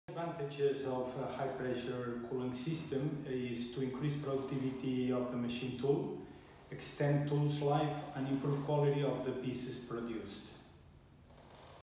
Las ventajas de los sistemas refrigerantes de SCS
Pere Armora, export manager de SCS, explica los beneficios de los sistemas de distribución de refrigerante para las máquinas-herramienta.